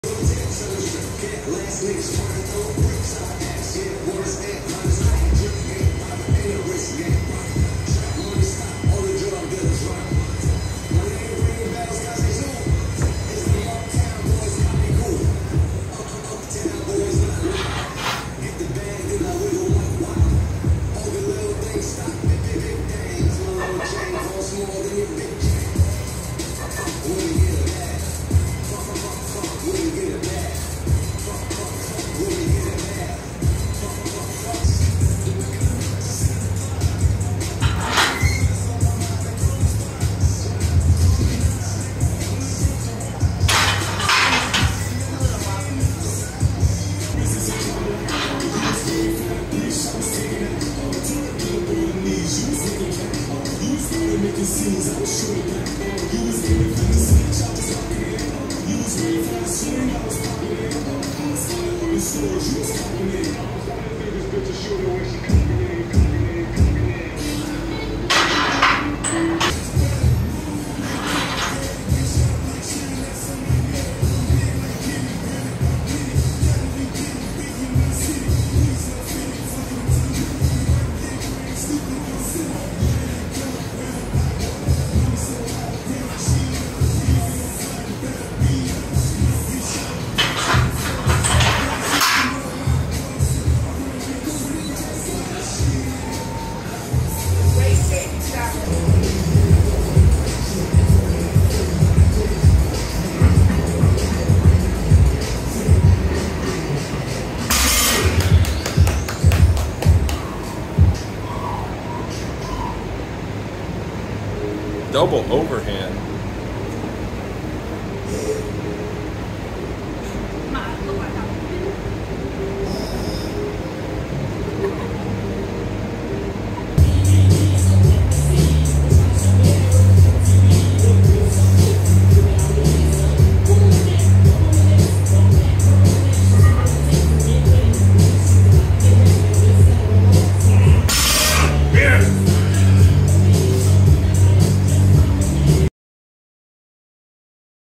Squats and Heavy Deadlifts - Deload Week?
Hit 285lbs for 5*5 aiming for a RPE 8. Then went to a 2" Deficit Deadlift for a top single. Hit a pretty easy 495lbs but failed a PR attempt of 550lbs.